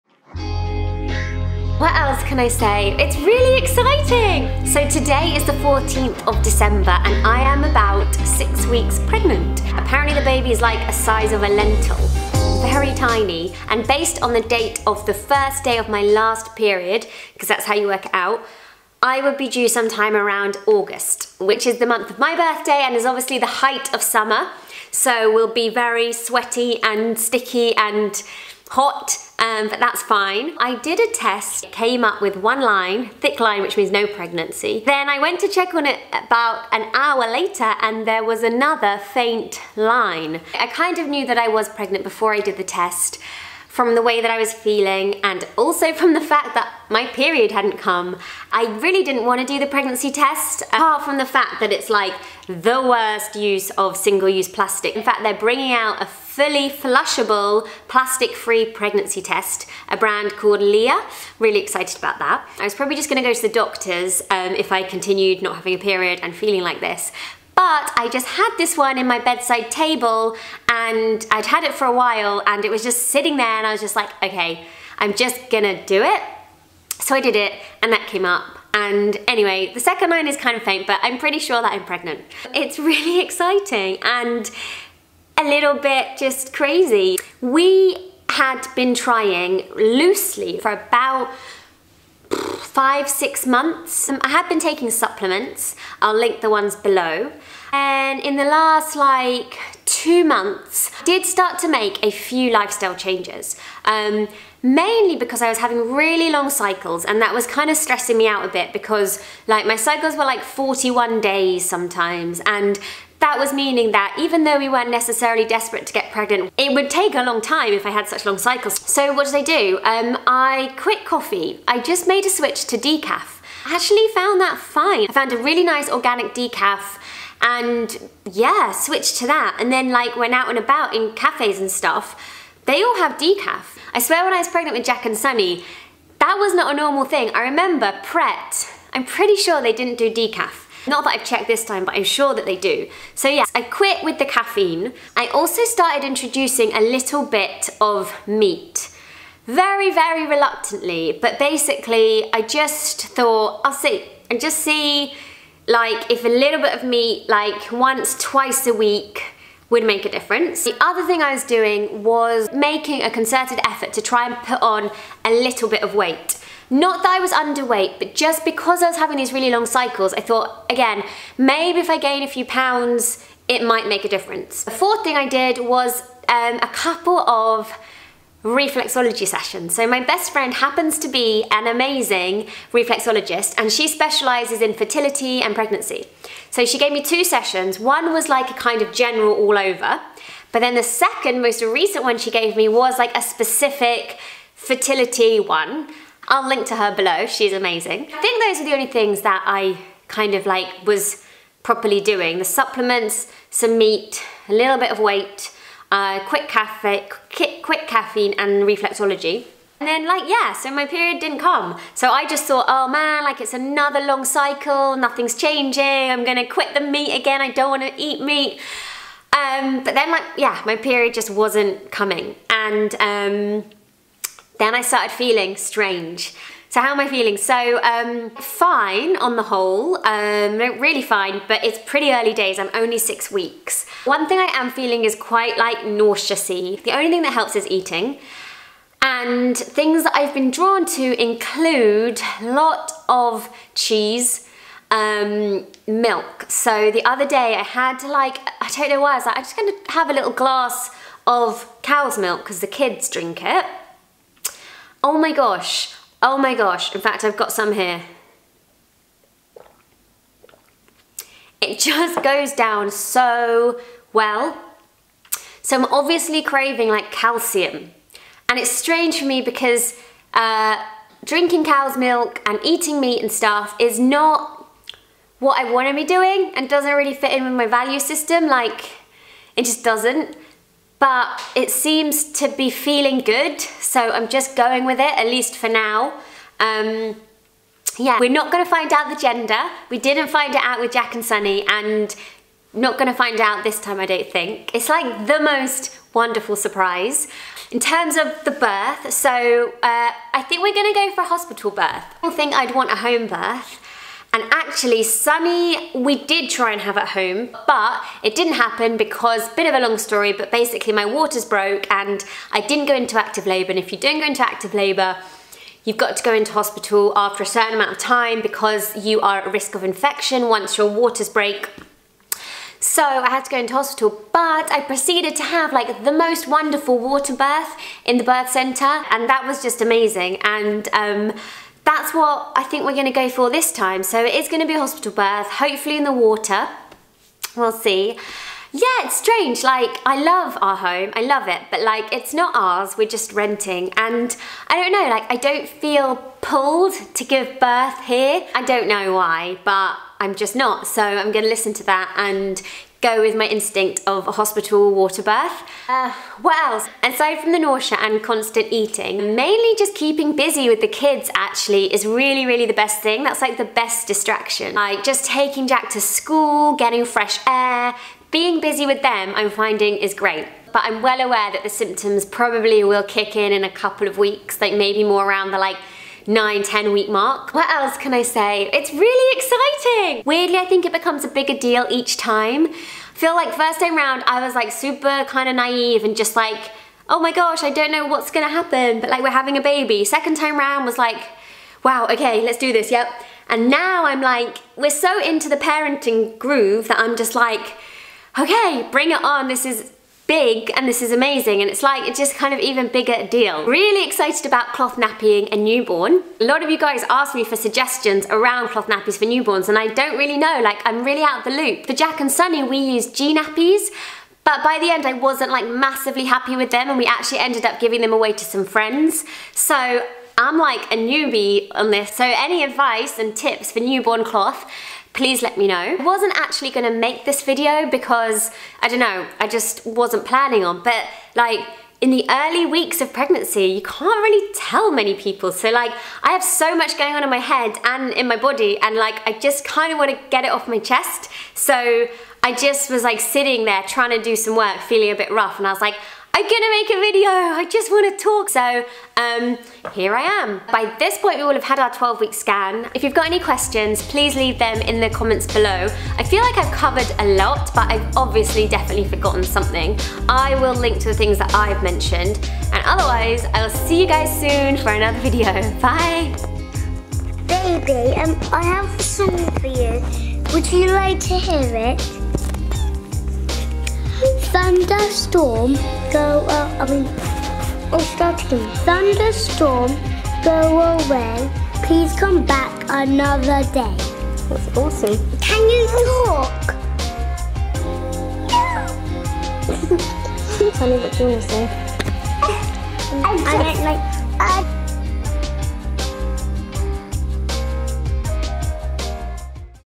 6 WEEKS PREGNANT: HOW I *THINK* I CONCEIVED, BIRTH PLANS & UNCOMFORTABLE CRAVINGS
Yup, I'm pregnant :) It's really exciting, and a little daunting. I plan to make a few of these videos through my pregnancy journey, so I hope you like them.

Please switch the toggle and watch this in HD 1080 - it looks waaay better.

Big love - E x

PLEASE WATCH THIS VIDEO IN HD!

Big love and high fives
x